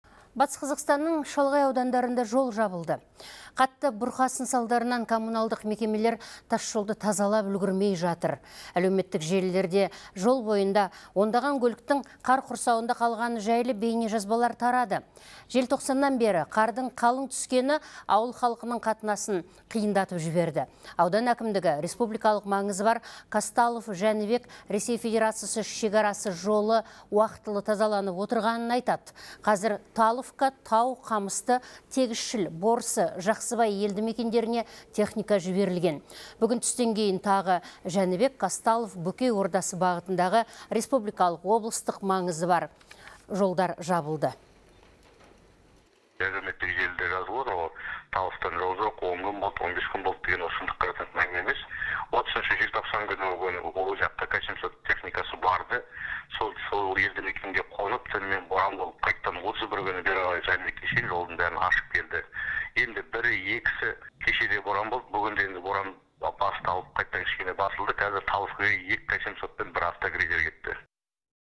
БҚО-ның шалғай аудандарында жол жабылды
Қатты бұрқасын салдарынан коммуналдық мекемелер тасжолды тазалап үлгермей жатыр. Әлеуметтік желілерде жол бойында ондаған көліктің қар құрсауында қалғаны туралы бейнежазбалар тарады. Желтоқсаннан бері қардың қалың түскені ауыл тұрғындарының қатынасын қиындатып жіберді. Аудан әкімдігі республикалық маңызы бар Казталов – Жәнібек - РФ шекарасы жолы уақытылы тазаланып отырғанын айтады. Қазір Таловка, Тау, Қамысты, Тегісшіл, Борсы, Жақсыбай елді мекендеріне техника жіберілген. Бүгін түстен кейін тағы Жәнібек, Казталов, Бөкей ордасы бағытындағы республикалық, облыстық маңызы бар жолдар жабылды.